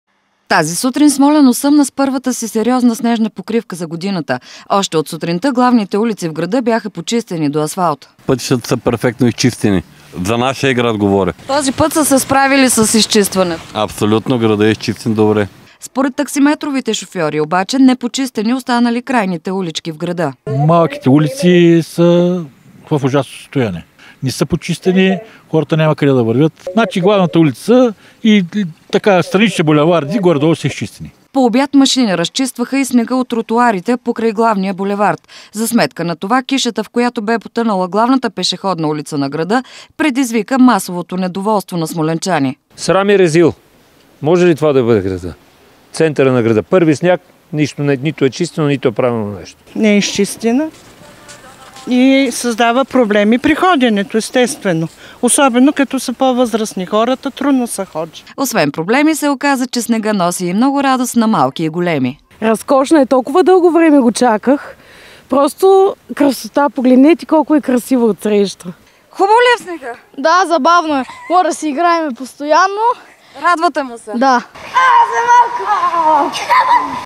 40 см. е снежната покривка в Смолян, хората недоволстват от непочистени улици
До 40 см достигна снежната покрива в Смолянска област. Снощи заради обилния снеговалеж няколко населени места в региона останаха за чaсове без електричество. Тази сутрин обстановката се нормализира. Няма въведени ограничения за движение на автомобили по проходите. В областния град обаче много хора негодуваха заради непочистените пешеходни алеи.